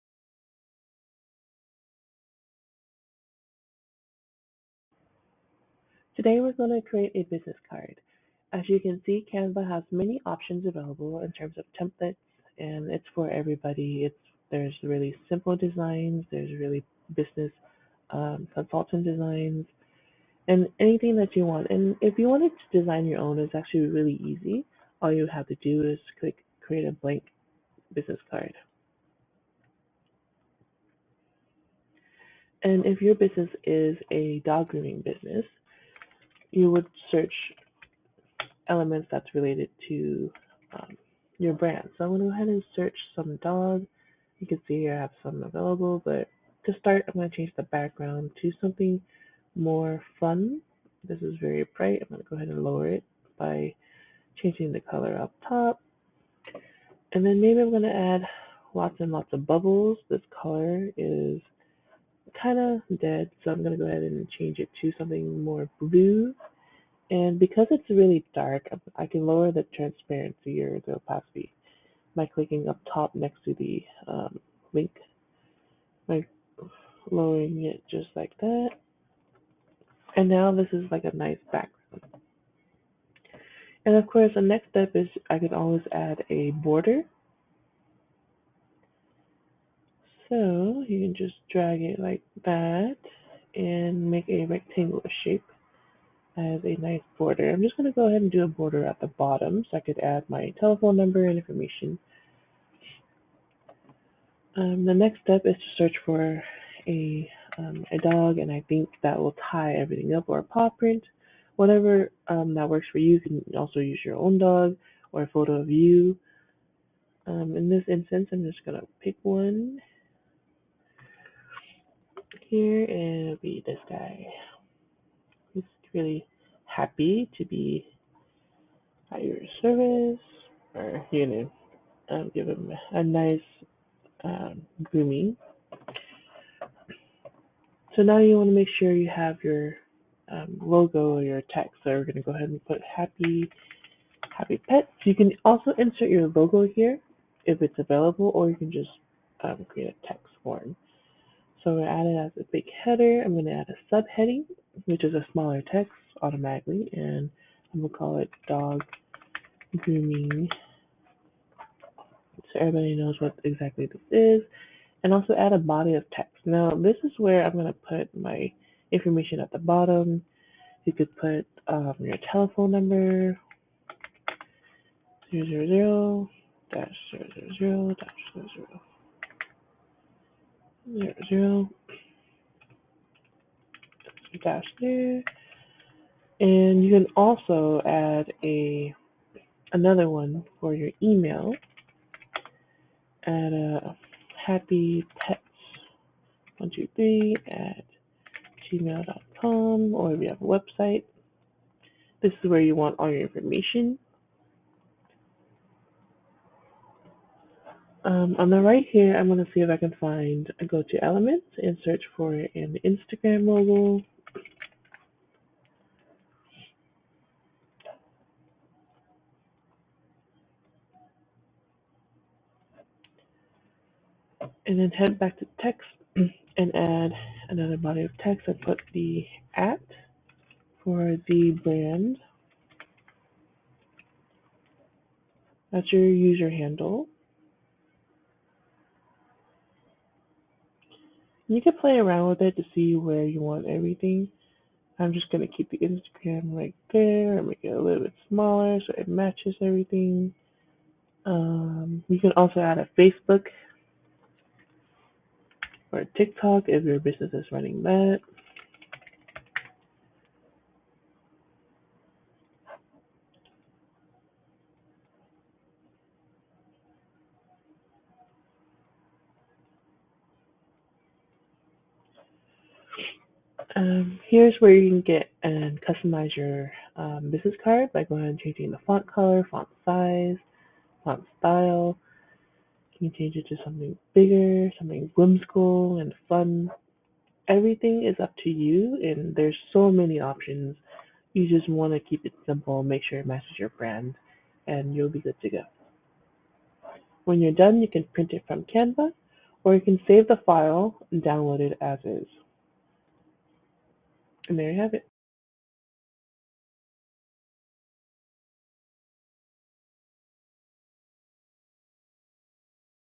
Business Card in Canva for FREE
In this video, I am going to show you how to design a business card in Canva for FREE. The example we're using today is for a dog grooming business. You can use my template below.